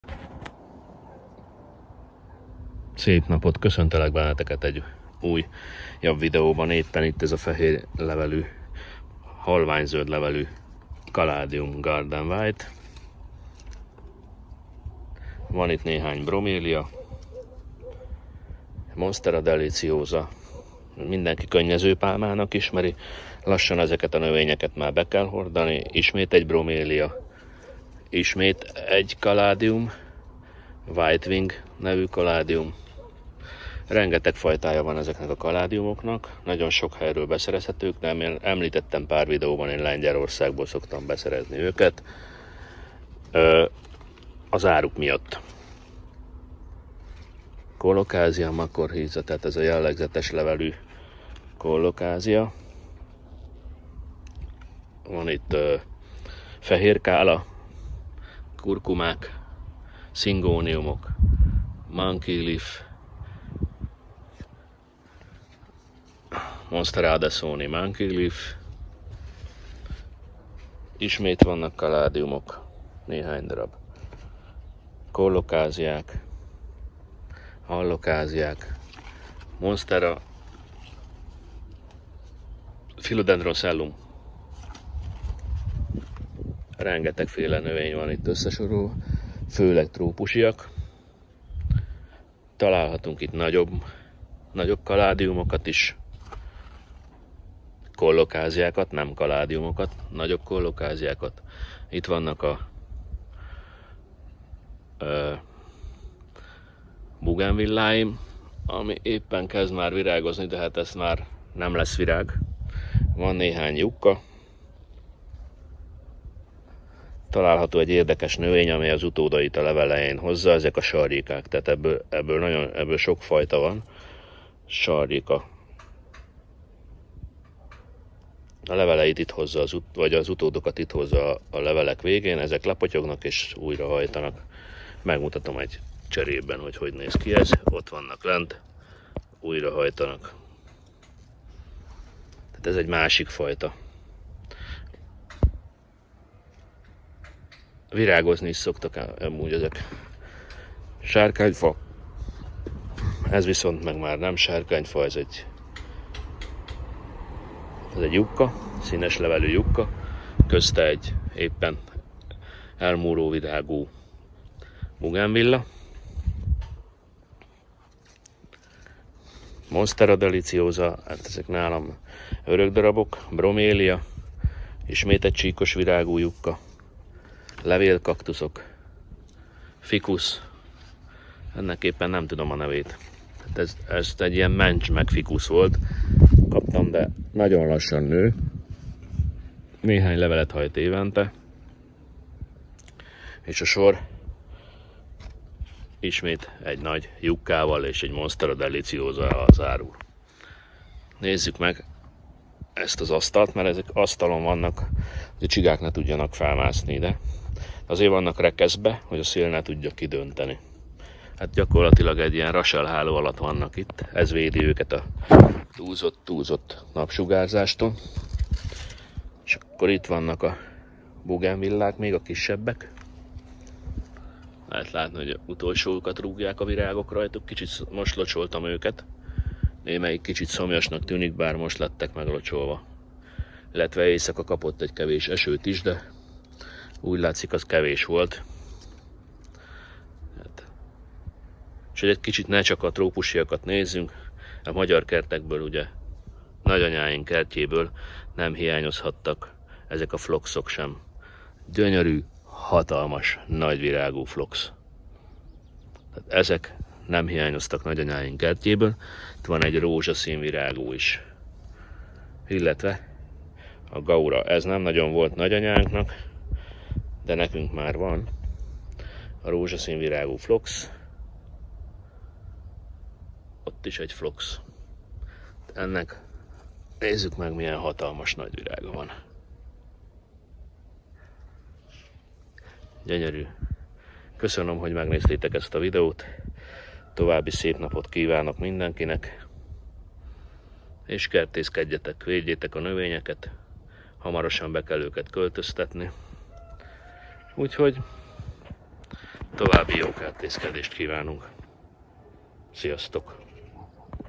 Ep. 118. Dísznövényeim 2.
Trópusi és mediterrán dísznövényeim bemutatása.